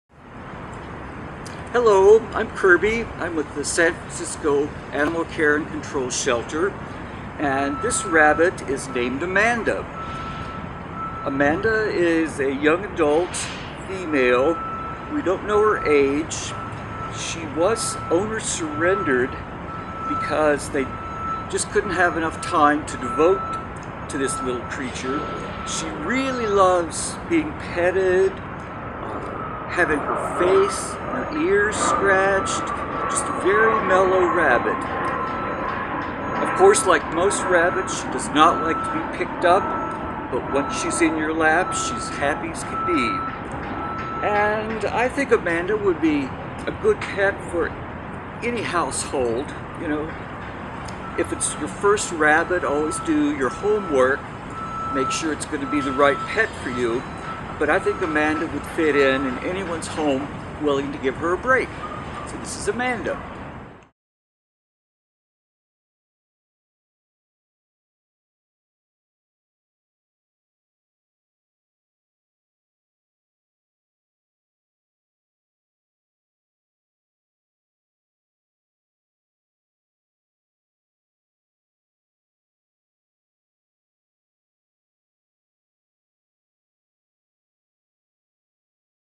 Amanda - A307310.mov
This pretty little girl is Amanda being introduced by longtime SFACC volunteer Kirby, AKA The Rabbit Whisperer. Amanda is a young adult female. She is an owner surrender because her humans didn't have time to care for her. She  loves being petted, and having her face and ears scratched; she is a very mellow girl. Like most rabbits, she doesn't like to be picked up, but once she is in your lap, she is as happy as she can be. Amanda would be a great addition to any household, even for first time rabbit adopters, provided they have done their homework and know what to expect and how to care for a rabbit.